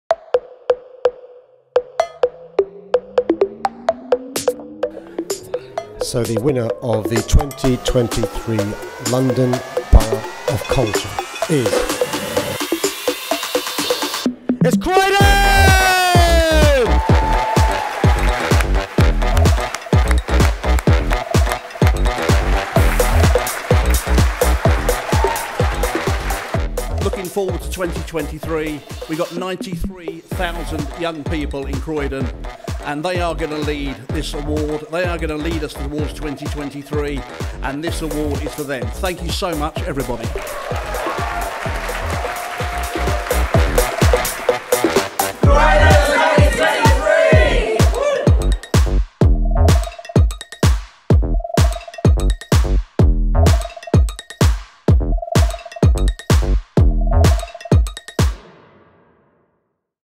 Croydon wins London Borough of Culture 2023
Mayor of London Sadiq Khan announced Croydon as the London Borough of Culture for 2023.